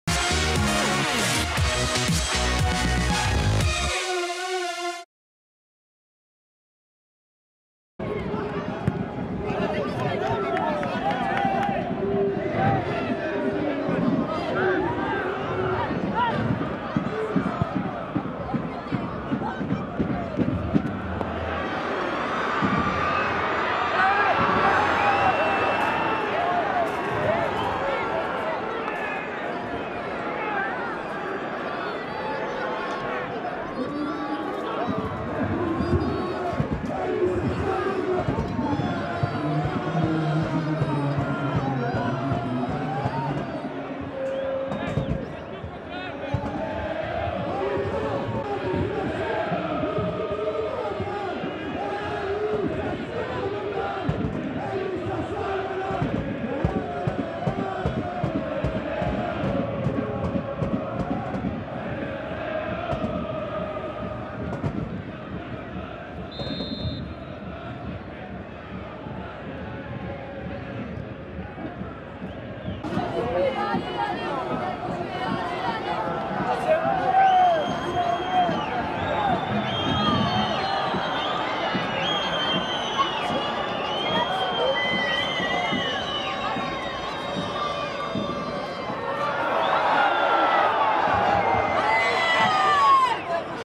But Lucas MAREQUE (75') - OGC Nice - FC Lorient (1-1) / 2012-13
OGC Nice - FC Lorient (1-1) - But de Lucas MAREQUE (75'). Tous les buts de OGC Nice - FC Lorient en vidéo.

Stade du Ray - samedi 9 février 2013

Buteurs : Lucas MAREQUE (75' - FC Lorient) / Valentin EYSSERIC (82' - OGC Nice)